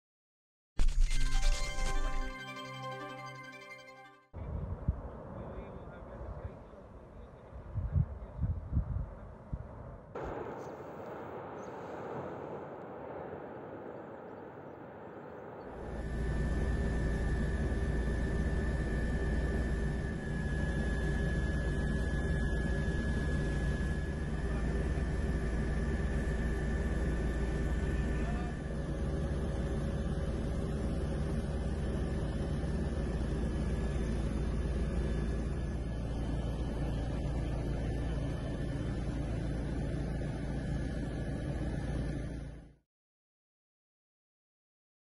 Lluvia de piroclastos y cenizas en La Palma
Las imágenes del avance de la erupción volcánica siguen generando expectación entre locales y visitantes.